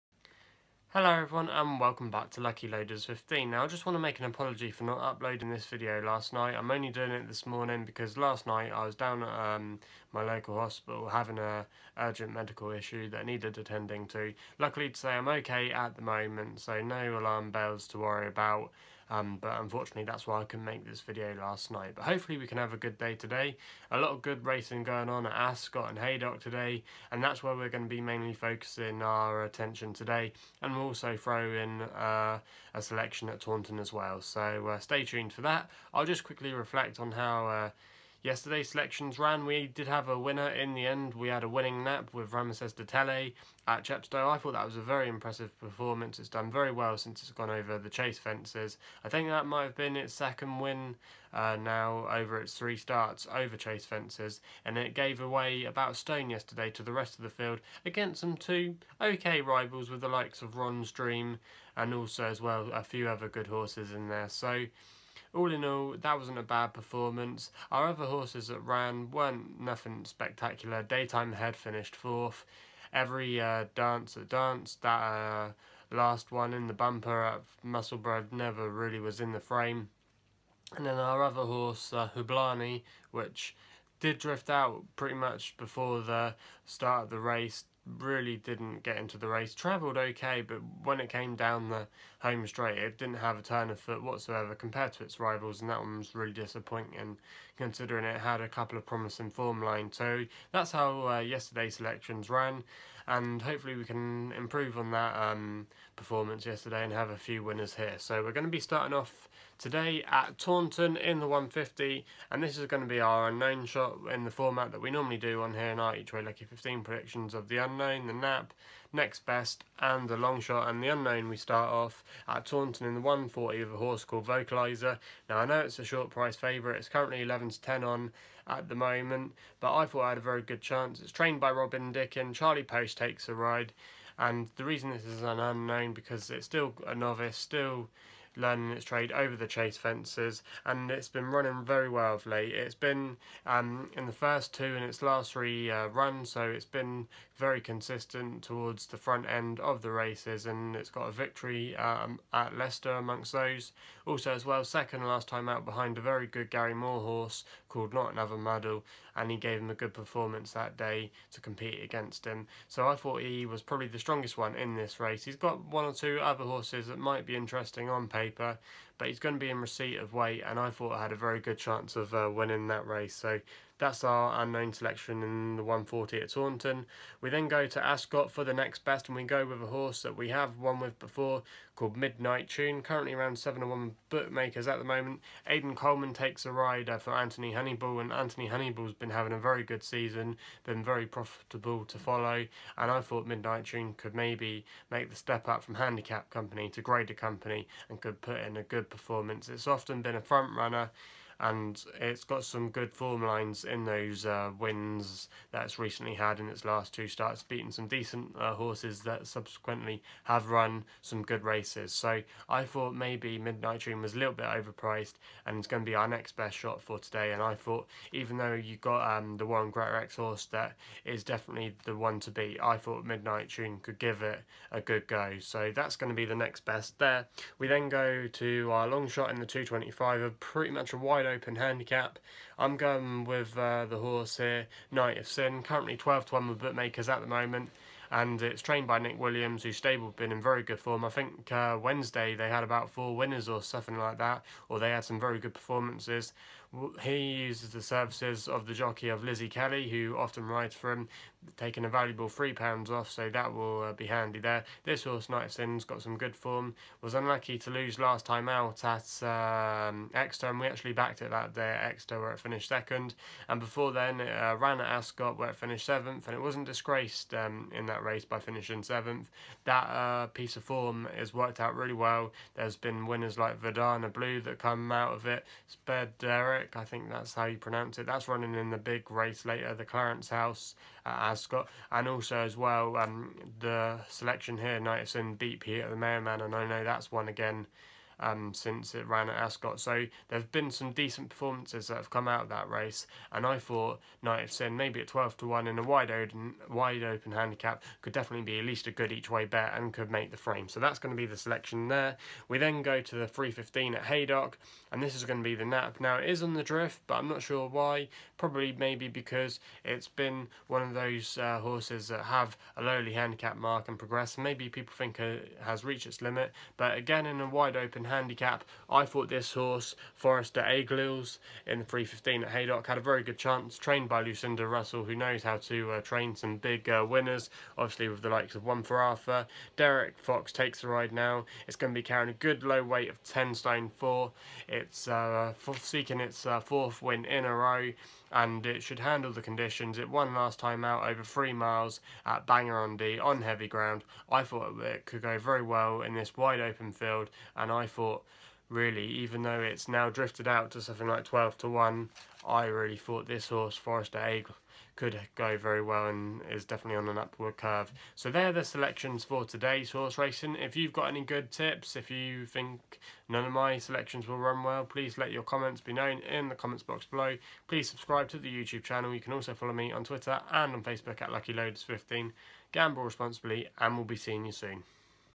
A Variety of Odds! Taunton, Ascot and Haydock EW Lucky15 Horseracing Tips 20th January 2018
Taunton 1.40 - Vocaliser 11/10 (Unknown)
Ascot 1.50 - Midnight Tune 7/1 (NB)
Ascot 2.25 - Night of Sin 12/1 (Longshot)
Haydock 3.15 - Forest Des Aigles 12 (NAP)